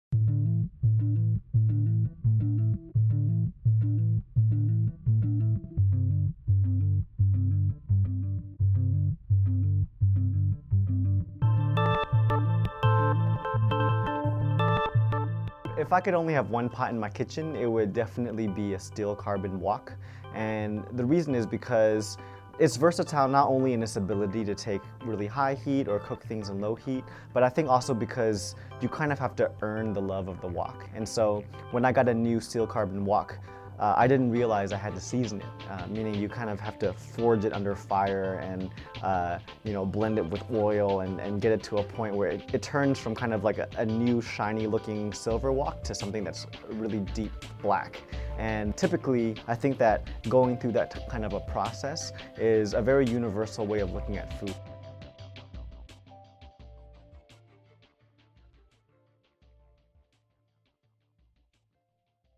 SIQ If you could only have one pot in your kitchen, which should you choose?
Adriel Luis, Curator of Digital & Emerging Media from Smithsonian's Asian Pacific American Center, answers: If you could only have one pot in your kitchen, which should you choose?